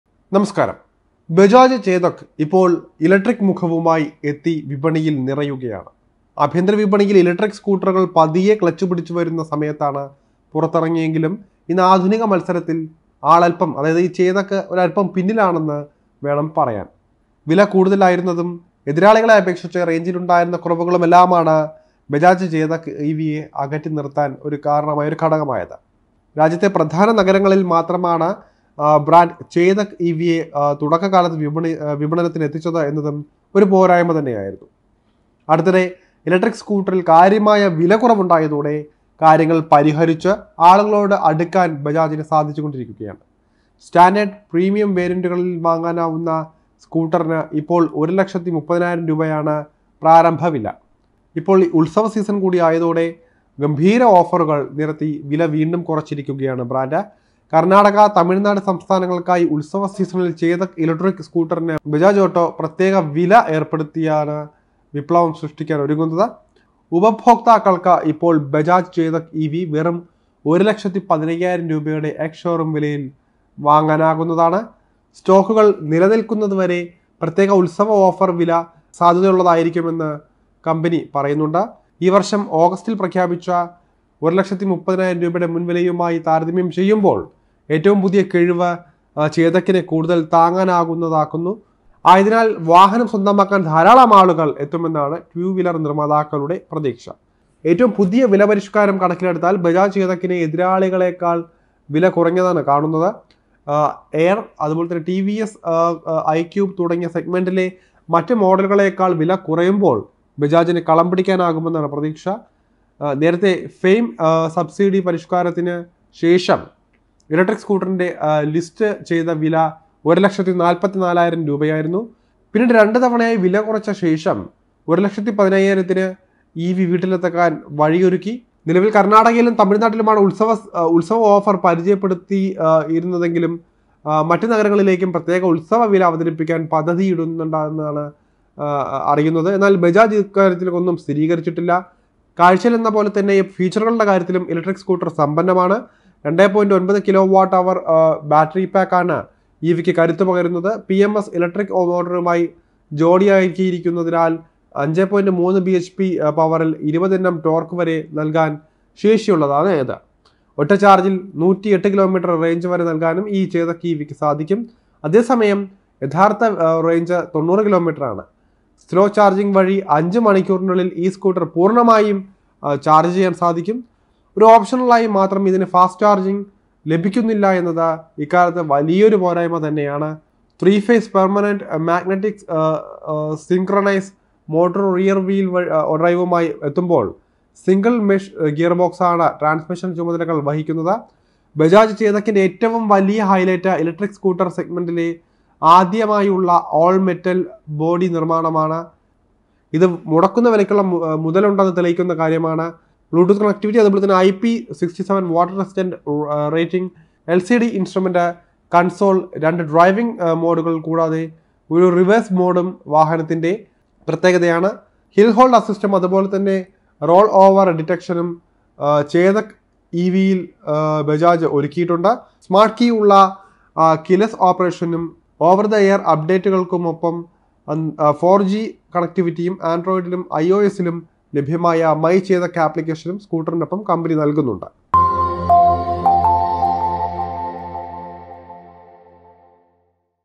ചേതക്കിന്റെ വില കുറച്ച് ബജാജ് | Bajaj Chetak Electric
ഇലക്ട്രിക് ചേതക്
വാങ്ങാൻ പറ്റിയ സമയം!
വെറും 1.15 ലക്ഷത്തിന്
കിട്ടും